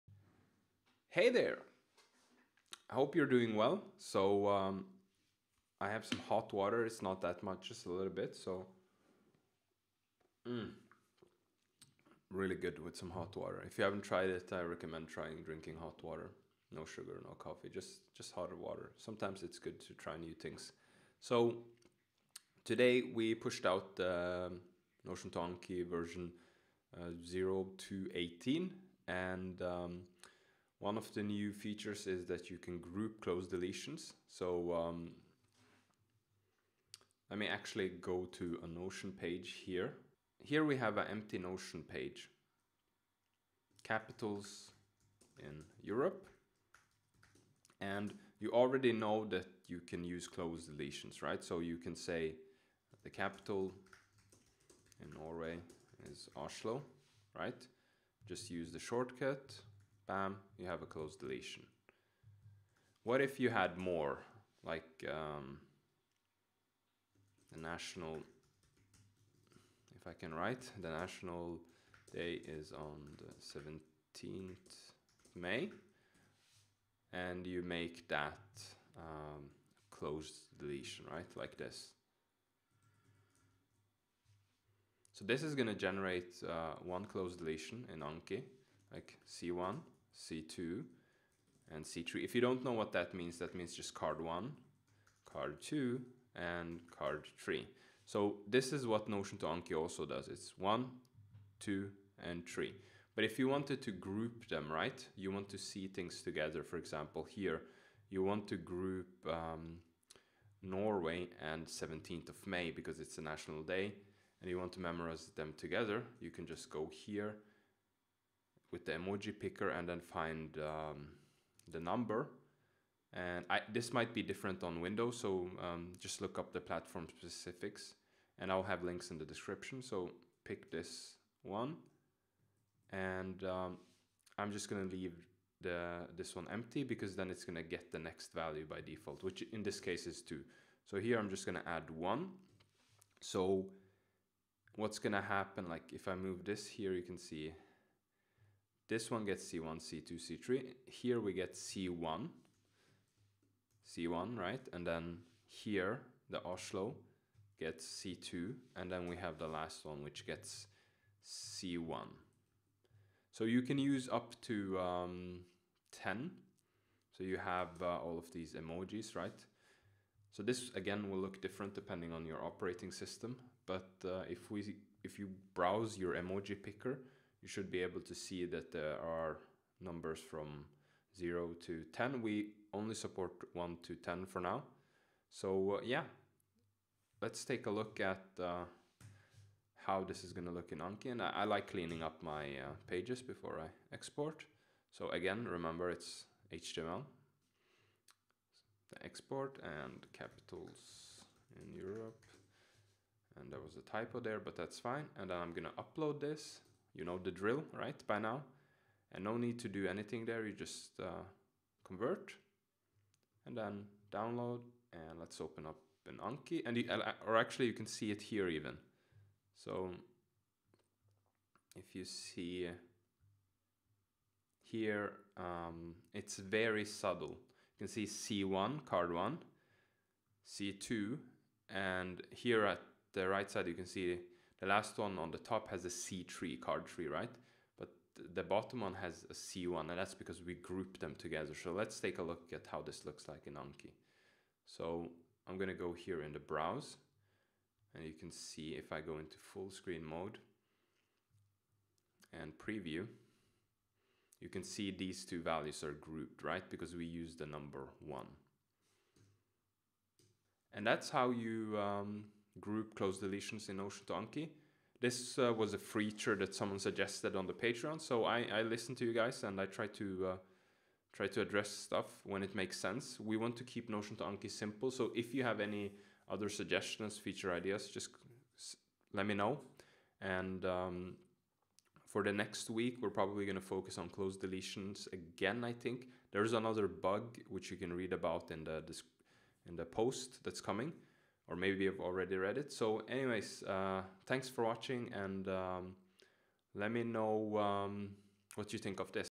What's cooking in notion2anki v0.2.18
In this video we look at the newly added support for grouping cloze deletions